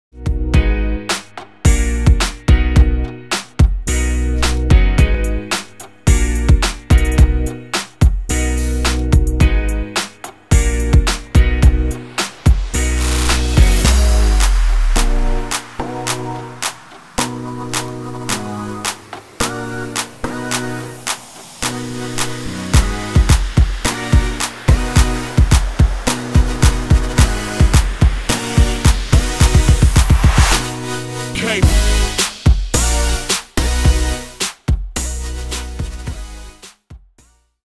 Elegant Dentistry - Photoshoot BTS
Go behind the scenes at a client photo session!